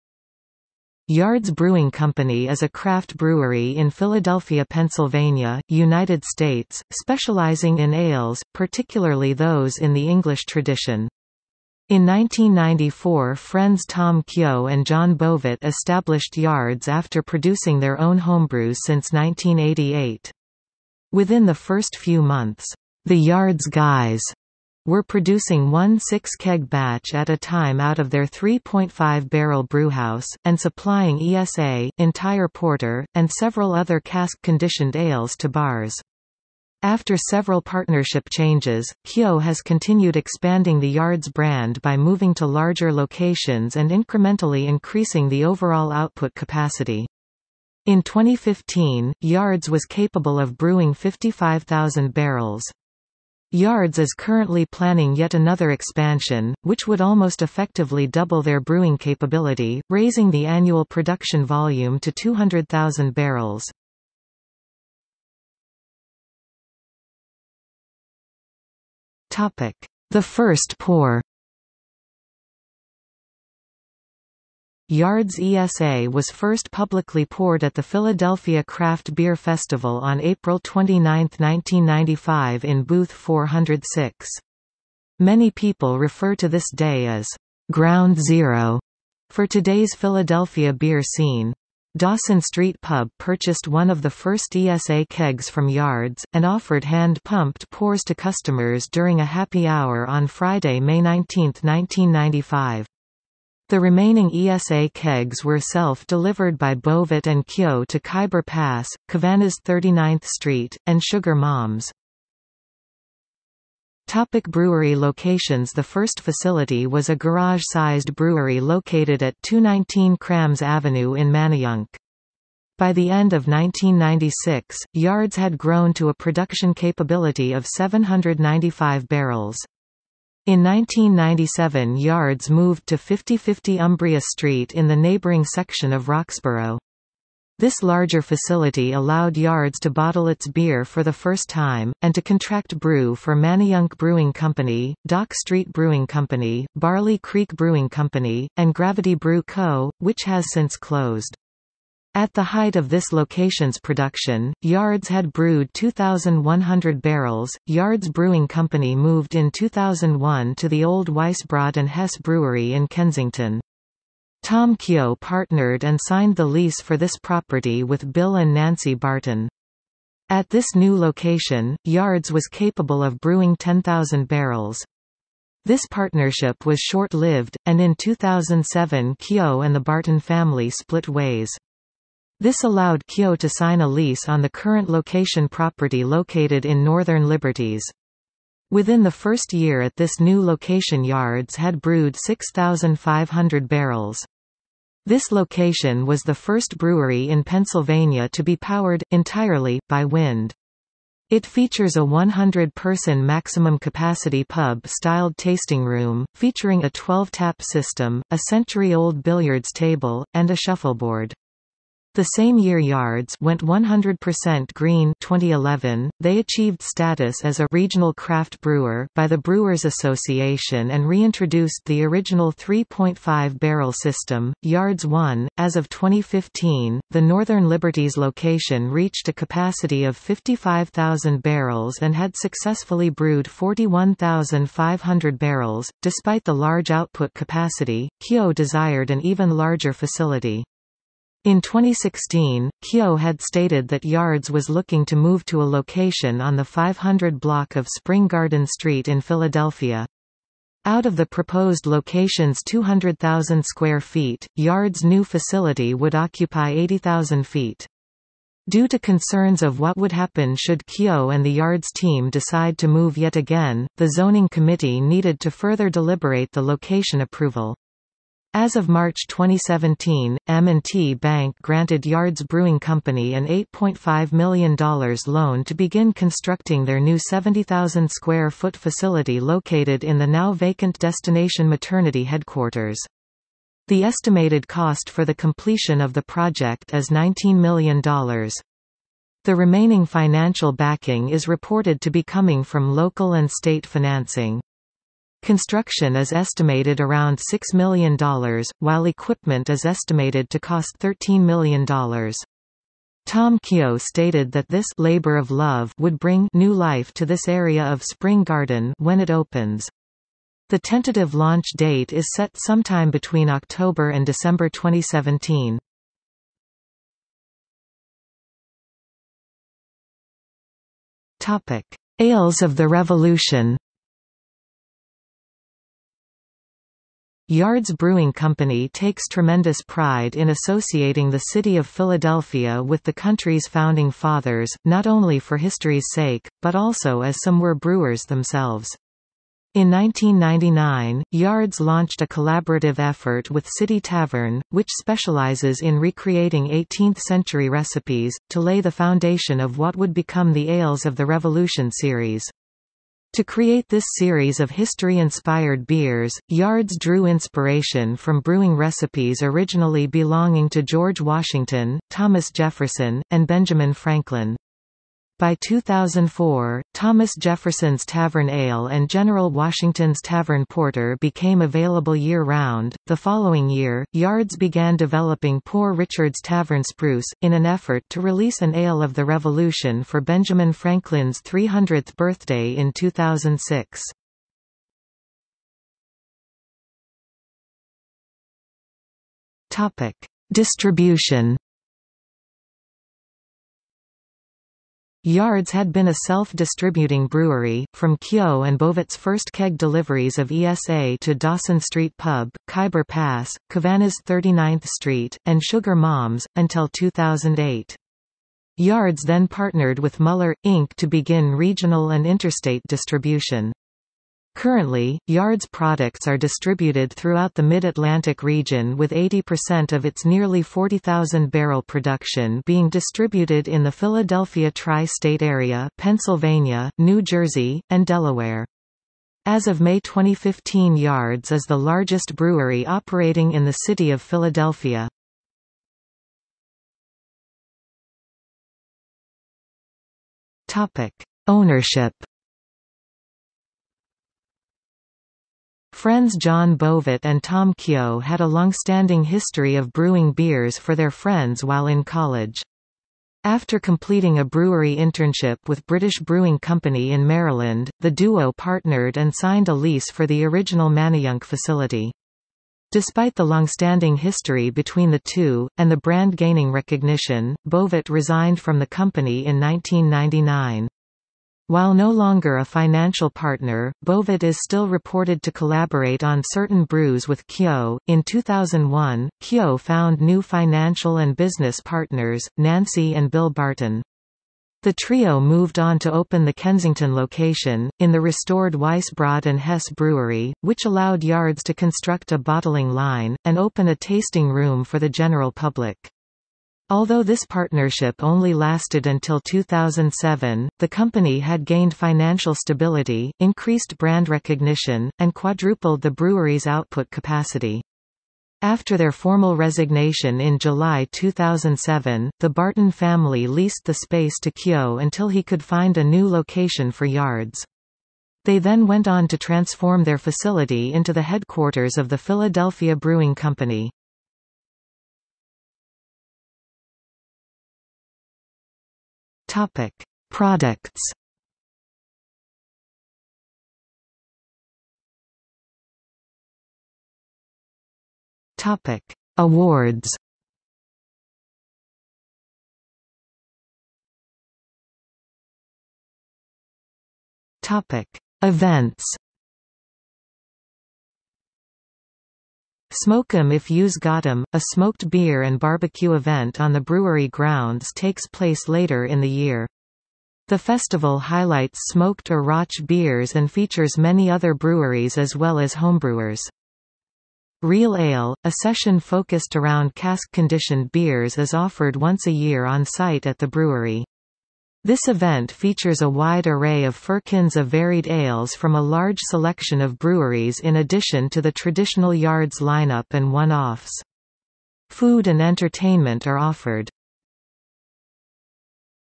Yards Brewing Company | Wikipedia audio article
00:01:05 1 The first pour
00:01:53 2 Brewery locations
00:05:17 3 Ales of the Revolution
00:06:26 4 Distribution
00:07:17 5 Ownership
00:08:45 6 Products
00:08:55 7 Awards
00:09:04 8 Events

- Socrates

SUMMARY
Yards Brewing Company is a craft brewery in Philadelphia, Pennsylvania, United States, specializing in ales, particularly those in the English tradition.  In 1994 friends Tom Kehoe and Jon Bovit established Yards after producing their own homebrews since 1988. Within the first few months, "the Yards Guys" were producing one six-keg batch at a time out of their 3.5-barrel brewhouse, and supplying ESA, Entire Porter, and several other cask-conditioned ales to bars. After several partnership changes, Kehoe has continued expanding the Yards brand by moving to larger locations and incrementally increasing the overall output capacity.  In 2015, Yards was capable of brewing 55,000 barrels.  Yards is currently planning yet another expansion, which would almost effectively double their brewing capability, raising the annual production volume to 200,000 barrels.